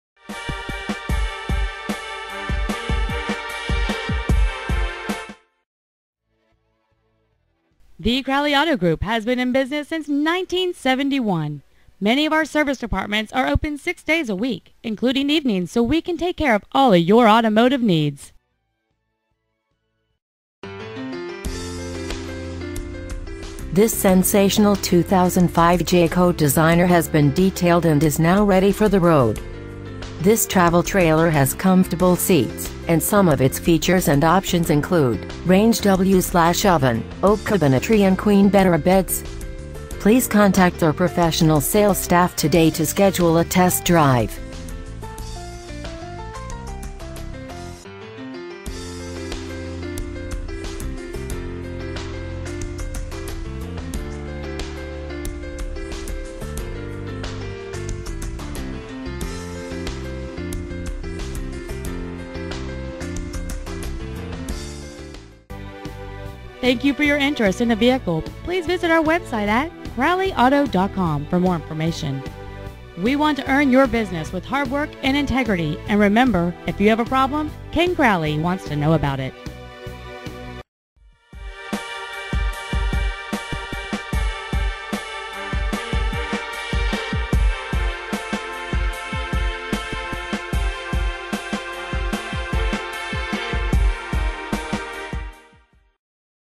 Pre-Owned 2005 Jayco Designer Bristol CT 06010
Year: 2005
 Make: Jayco
 Model: Designer
 Engine: L NOT SPECIFIED Not Specified
 Trans.: Not Specified
 Exterior: White

 Interior: Tan